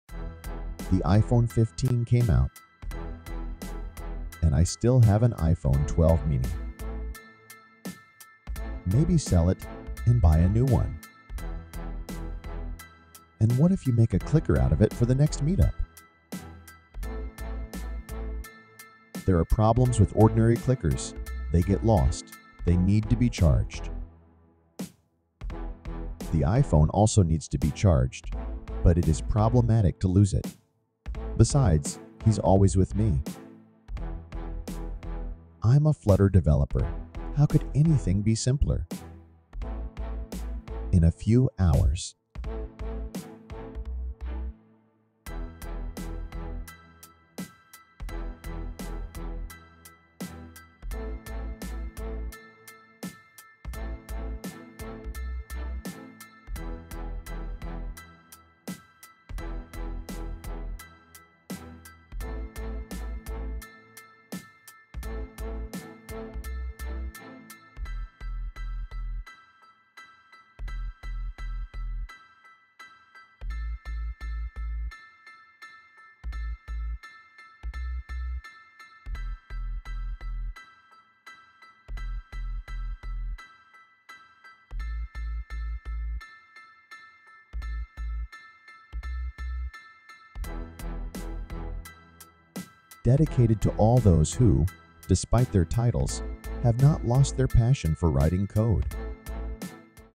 Mastering presentation control with the iPhone: Meet the Flutter Clicker
The most expensive clicker for presentations.
The iPhone 15 came out, and I still have an iPhone 12 mini. Maybe sell it and buy a new one.
And what if you make a clicker out of it for the next meetup.
There are problems with ordinary clickers, they get lost, they need to be charged.
The iPhone also needs to be charged, but it is problematic to lose it. Besides, he's always with me.
I'm a Flutter developer, how could anything be simpler.
In a few hours.
Dedicated to all those who, despite their titles, have not lost their passion for writing code!

Flutter allows you to create multi-platform apps for Android, iOS( desktop and web).